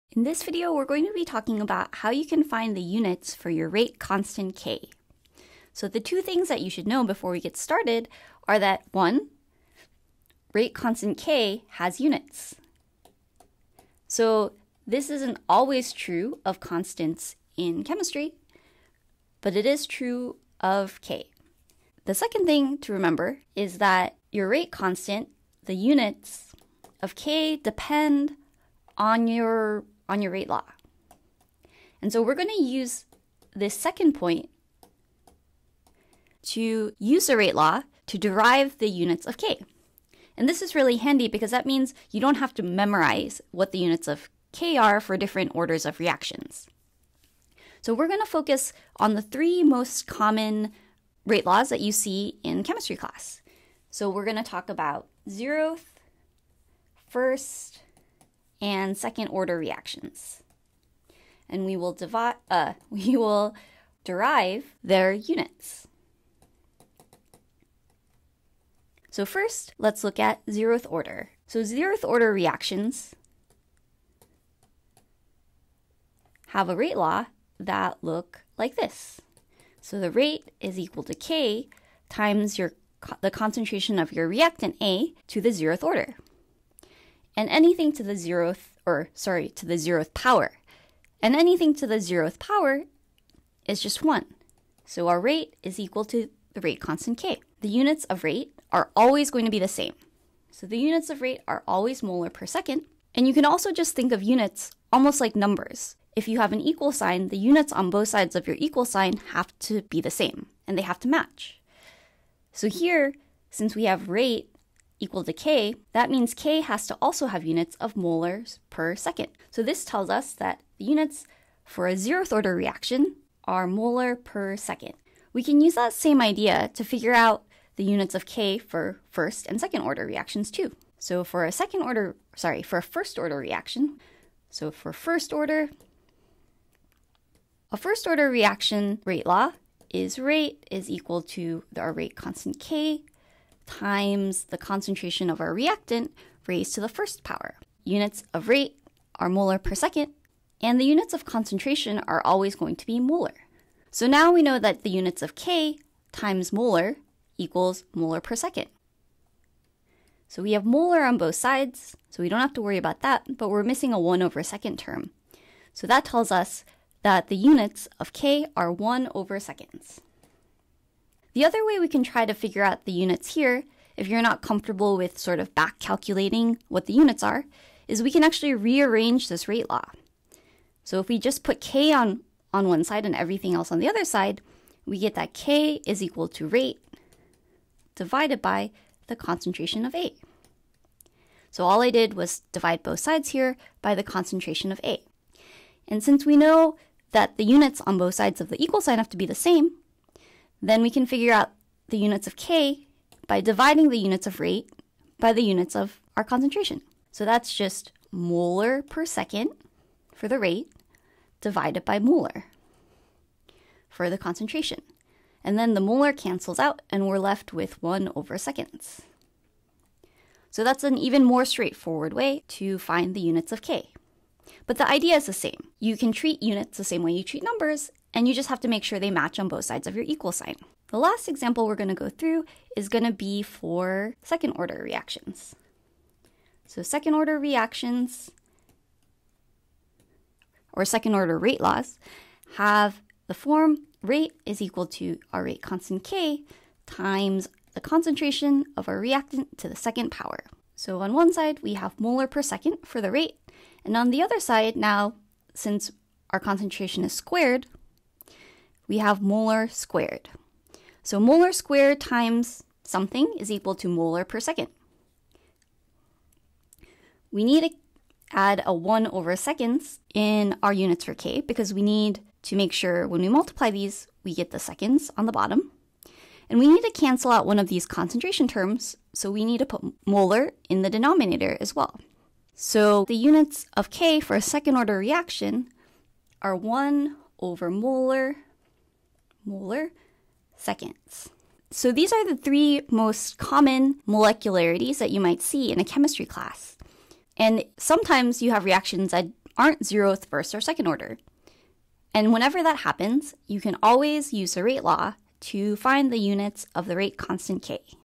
Units of the rate constant | Kinetics | AP Chemistry | Khan Academy
Courses on Khan Academy are always 100% free. Start practicing—and saving your progress—now

The units of the rate constant, k, depend on the overall reaction order. The units of k for a zero-order reaction are M/s, the units of k for a first-order reaction are 1/s, and the units of k for a second-order reaction are 1/(M·s). View more lessons or practice this subject